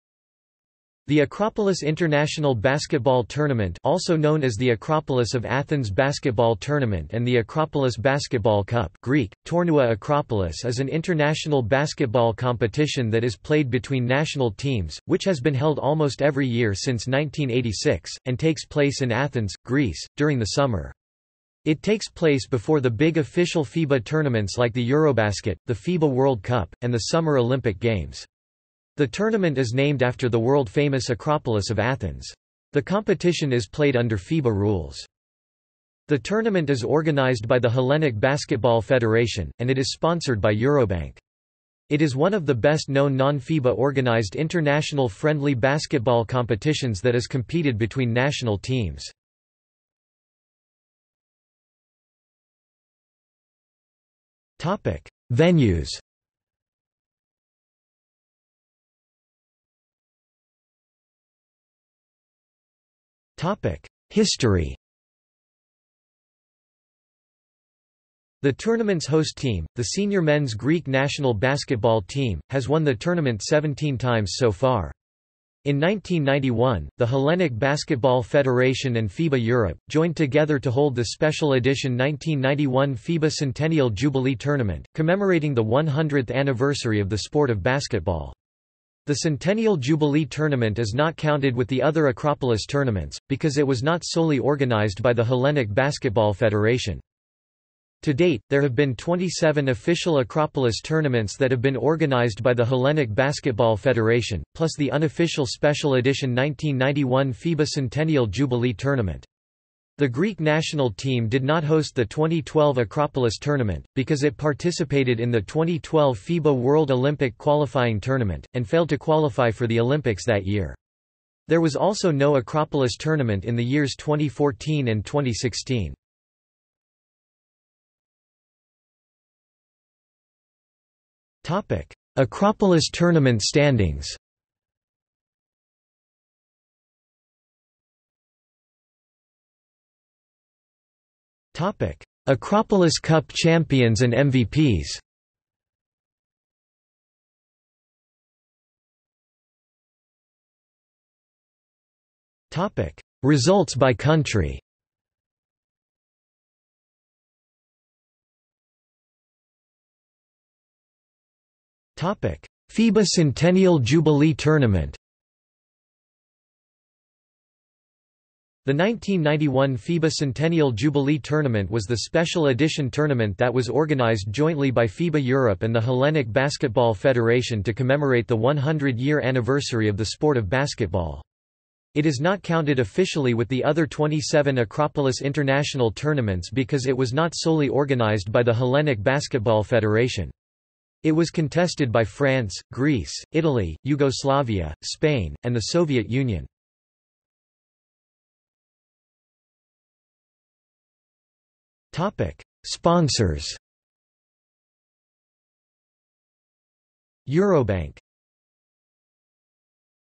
Acropolis International Basketball Tournament | Wikipedia audio article
00:00:51 1 Venues
00:00:59 2 History
00:02:07 3 Acropolis Tournament standings
00:02:17 4 Acropolis Cup Champions and MVPs
00:02:28 5 Results by country
00:02:38 6 FIBA Centennial Jubilee Tournament
00:03:18 7 Sponsors

- Socrates

SUMMARY
The Acropolis International Basketball Tournament (also known as the Acropolis of Athens Basketball Tournament and the Acropolis Basketball Cup) (Greek: Τουρνουά Ακρόπολις) is an international basketball competition that is played between national teams, which has been held almost every year since 1986, and takes place in Athens, Greece, during the summer. It takes place before the big official FIBA tournaments like the EuroBasket, the FIBA World Cup, and the Summer Olympic Games. The tournament is named after the world-famous Acropolis of Athens. The competition is played under FIBA rules.